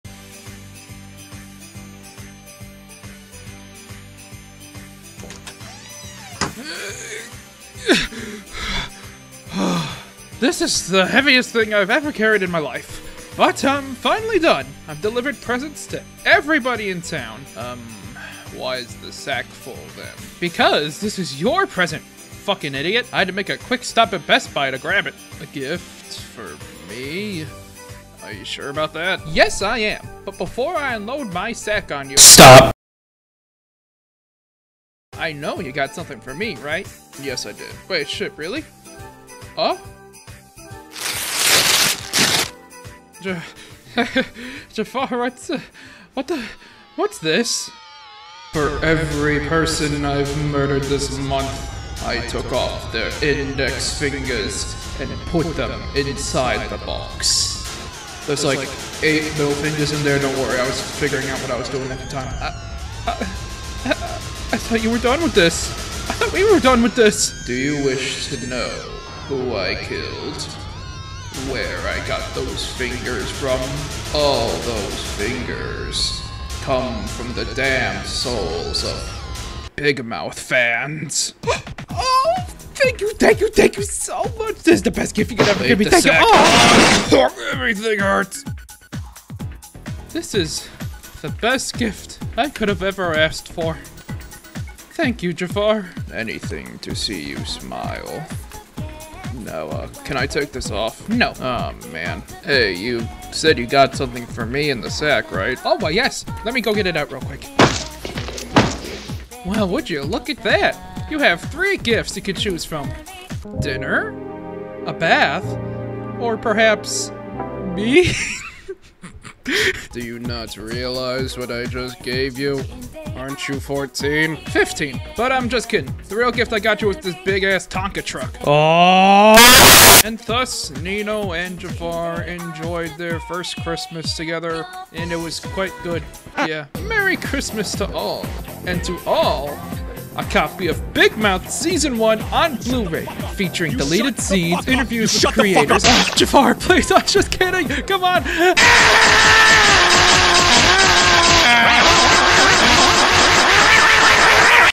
A Very Nino and Jaffar Christmas!
Nino and Jaffar celebrate their first Christmas! Jaffar isn't used to handing out gifts though... will Nino like what's in store for her?